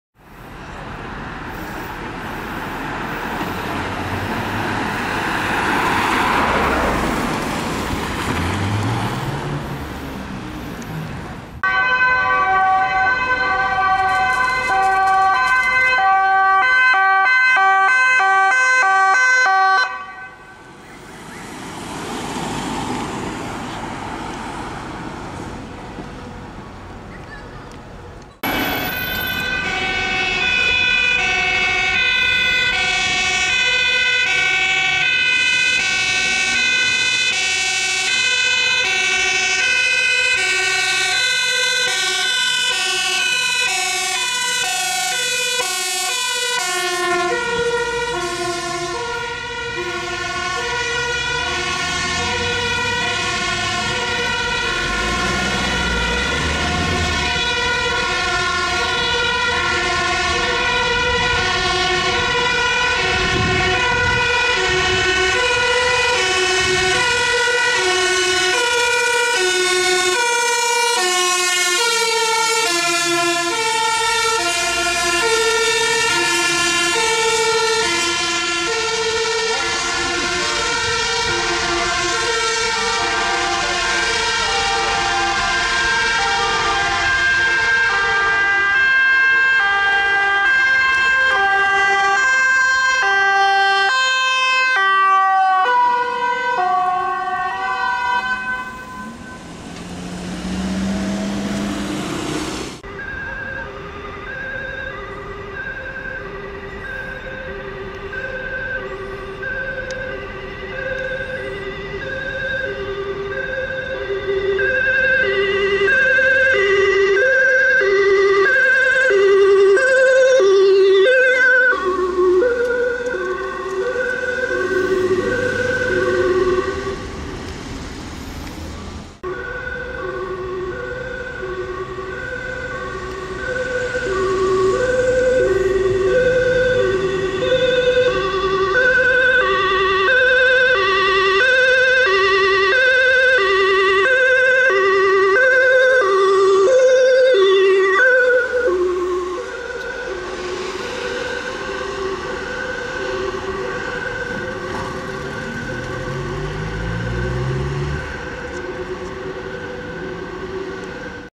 Hulpdiensten met spoed naar val van zesde etage Chopinplein Schiedam
©112Schiedam